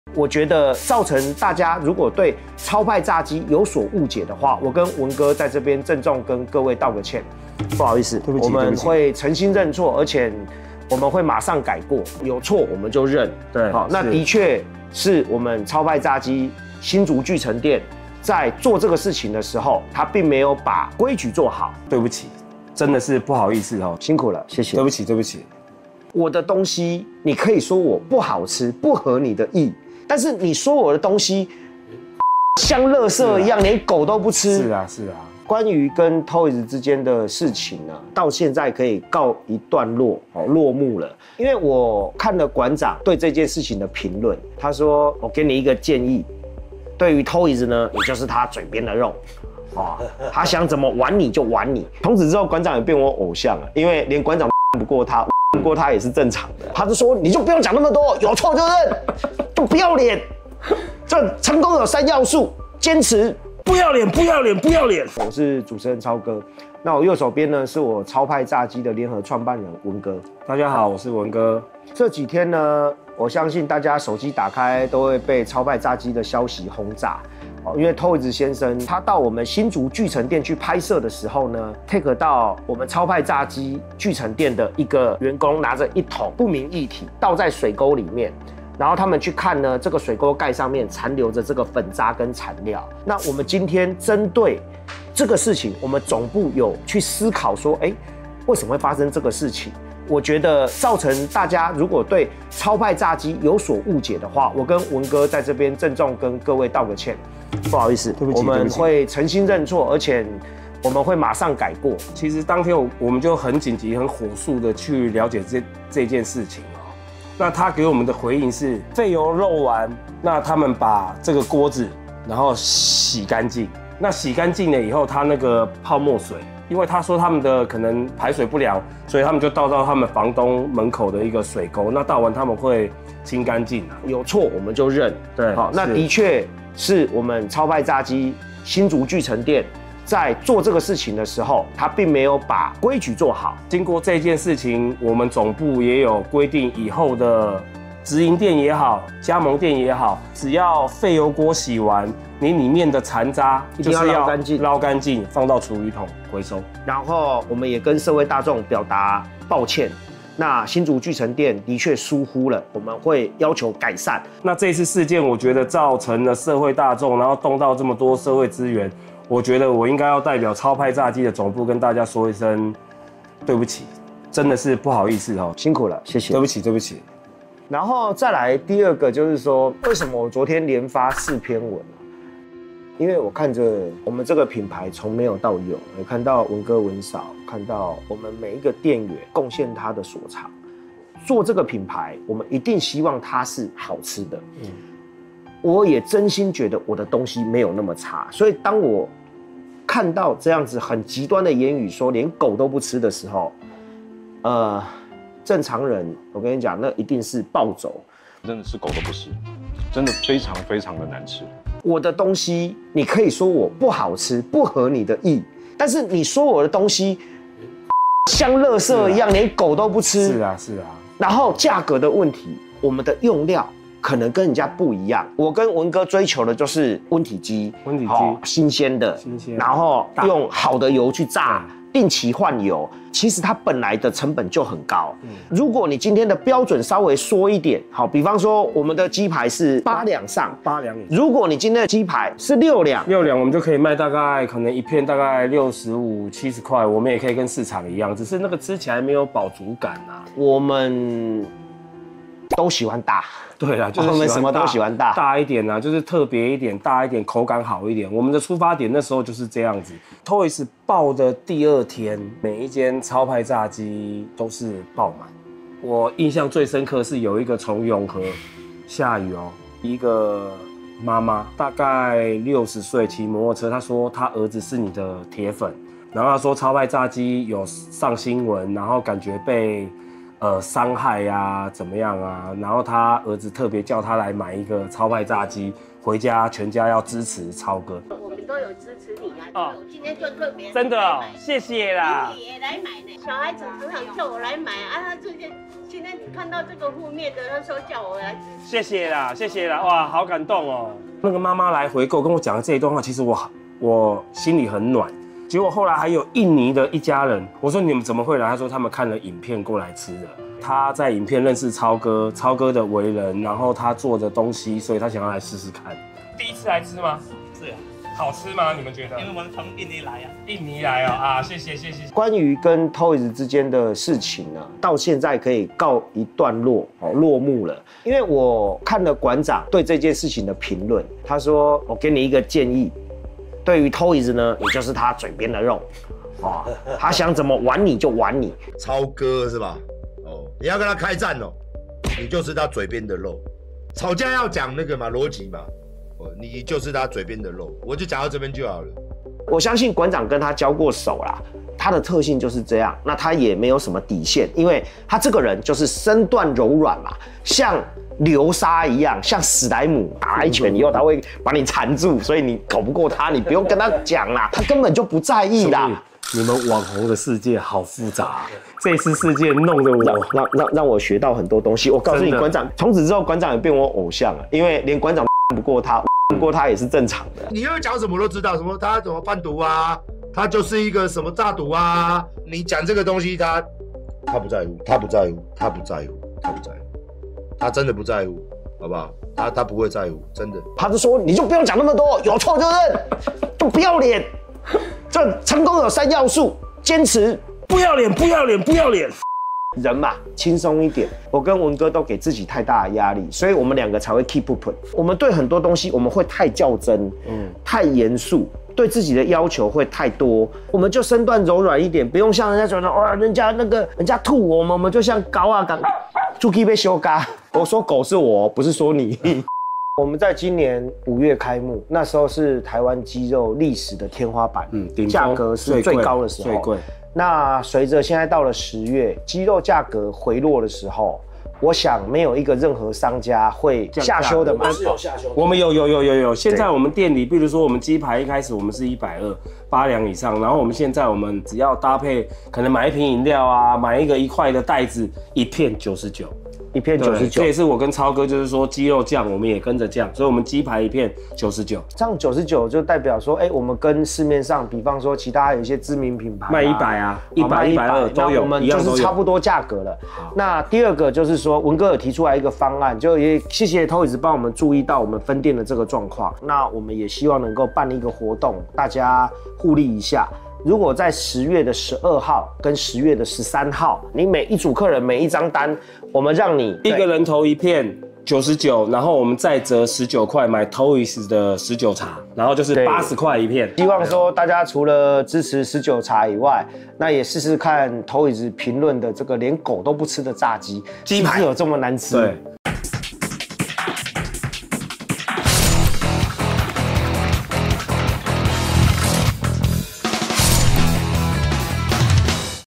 超哥直接道歉了！突上影片雙手合十曝「關鍵在館長」：有錯我們就認｜三立娛樂星聞
@superpie1111  授權